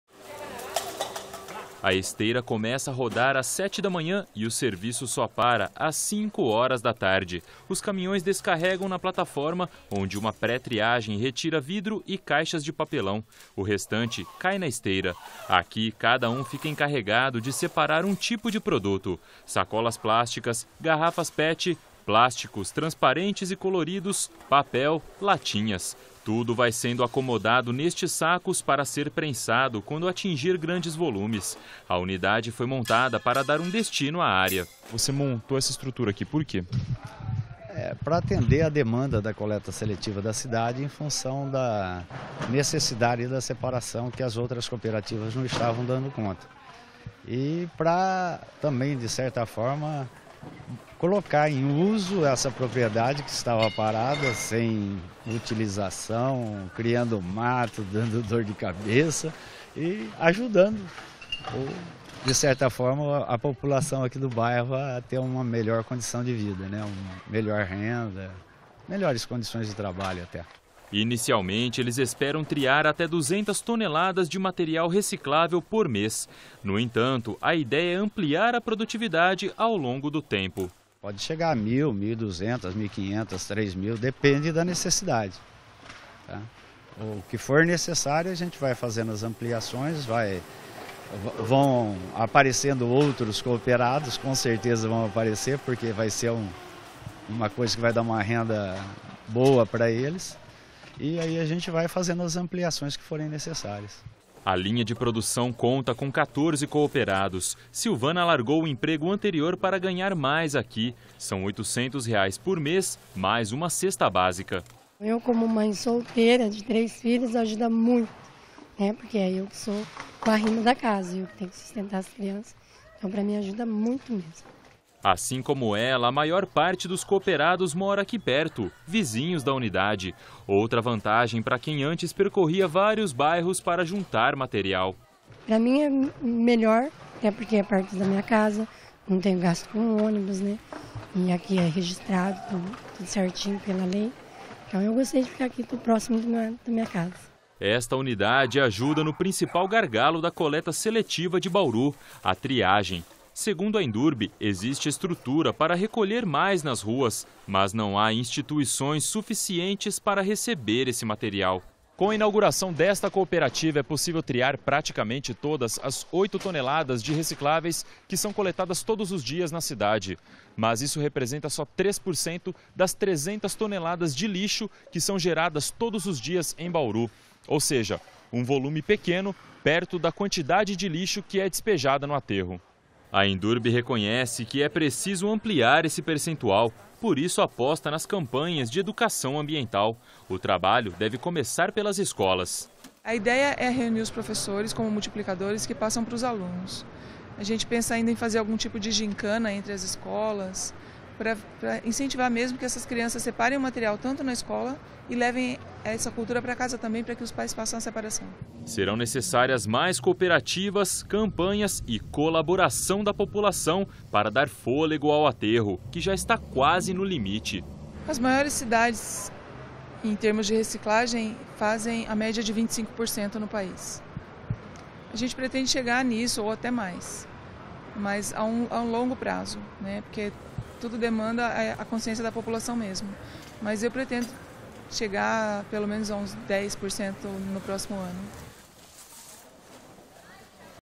Unesp Notícias - 18/03/2014 - Cooperativa inaugurada em Bauru vai ajudar na reciclagem de lixo - M
Começou a funcionar este mês a terceira cooperativa de reciclagem de Bauru. Com ela, será possível dar destino correto as mais de 8 toneladas de lixo reciclável produzidas todos os dias na cidade